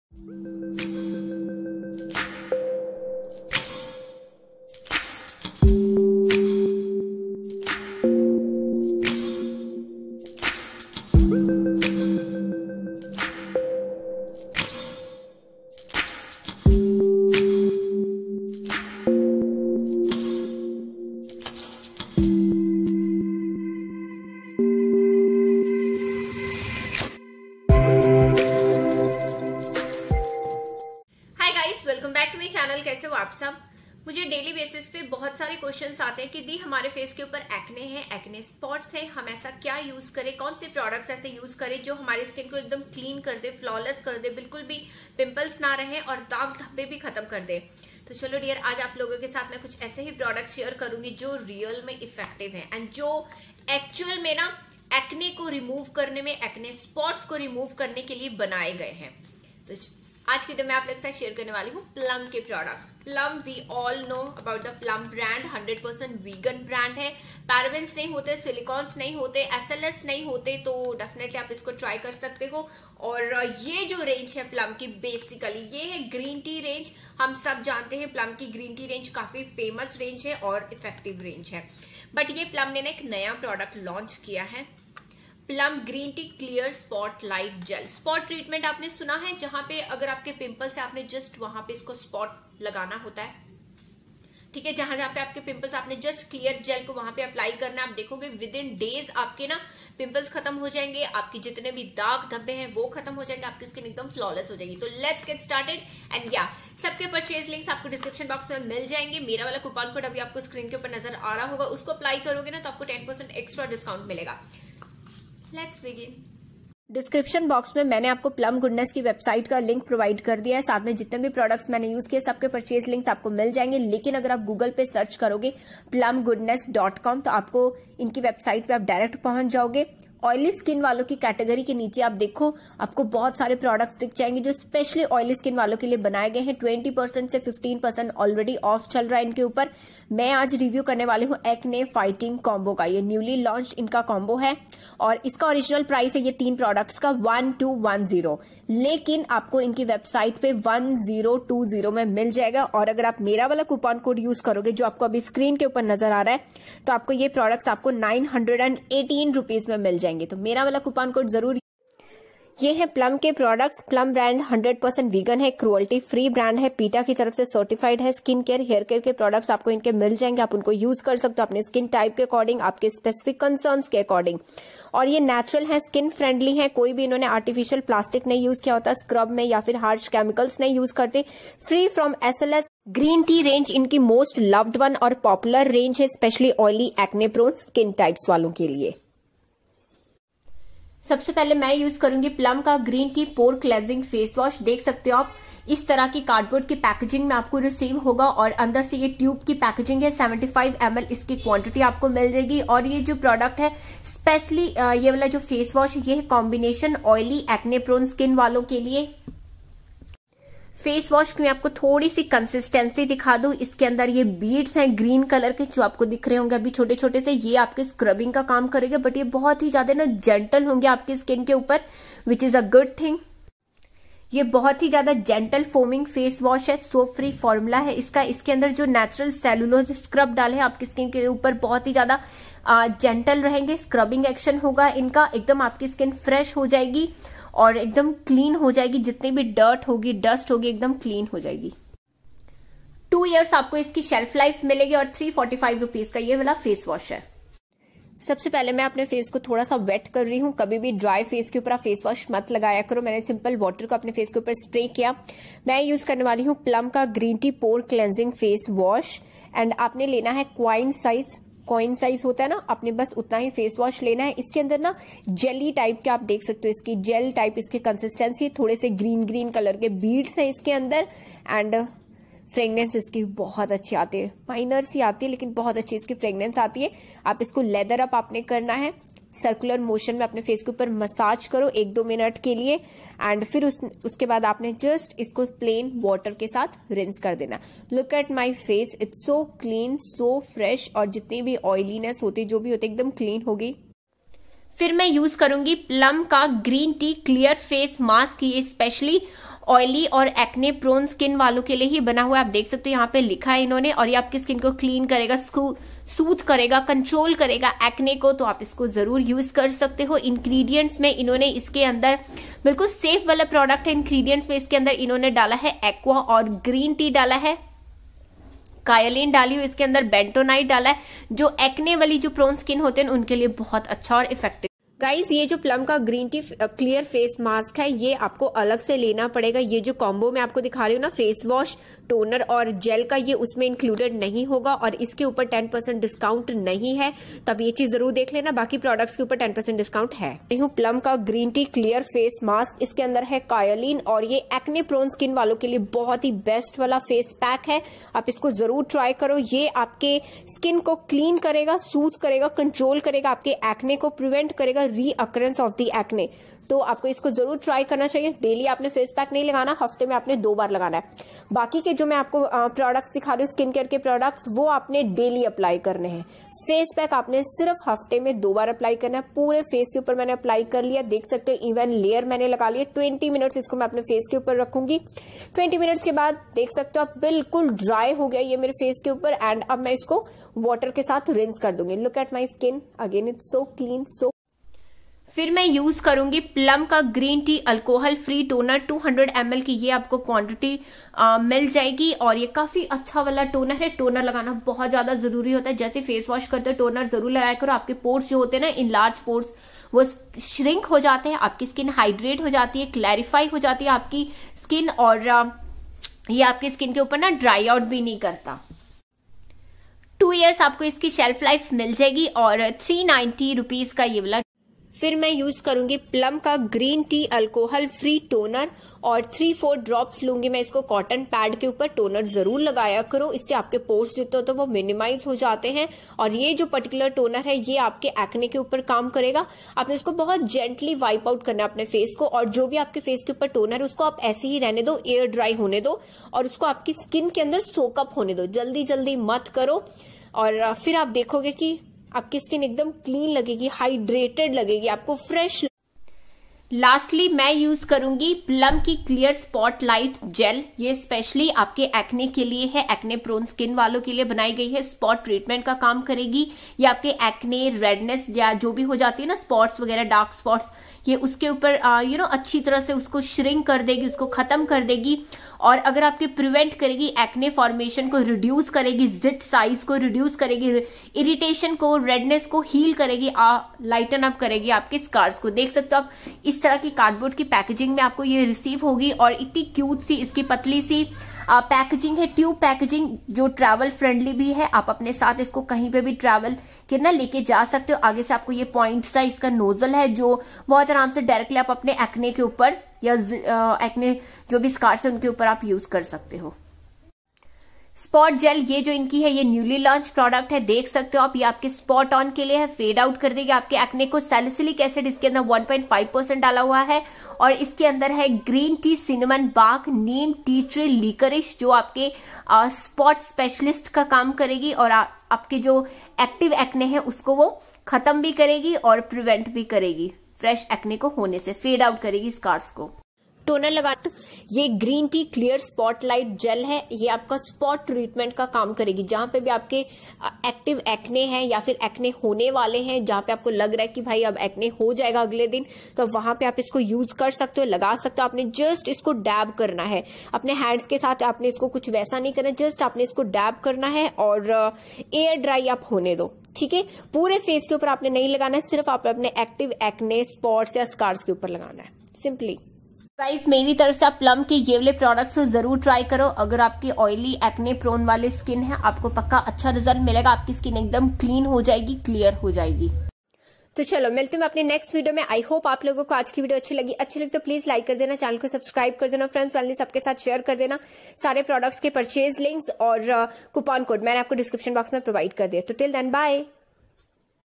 Solution for Acne & Acne spots - Plum’s new acne spot specialist /SWATI BHAMBRA
Step wise skin routine for those with acne and acne spots. 100% vegan products, free from SLS, parabens, phthalates, and harsh chemicals.

The MRP of the Green Tea Acne-fighting combo is Rs.  1,210. The combo price on Plum's website is Rs. 1,020. So you'll get it for just Rs. 918

👉Purchase Links:
Swati10

Products in focus: Green Tea: Face Wash, Toner, Clear Spot-Light Gel
Product sequence:
1. Face Wash
2. Toner
3. Clear Spot-Light Gel

Swati Bhambra - A Makeup Lover

 (SWATI BHAMBRA).